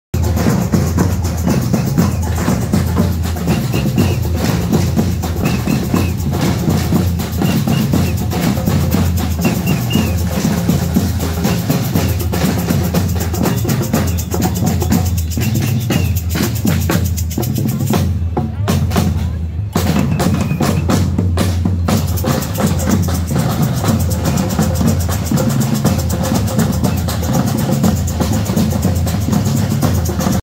Demo gegen die Corona-Maßnahmen in Bamberg am 07.02.2022
Die wunderbaren Trommler Gorleben sich zum Start der Demo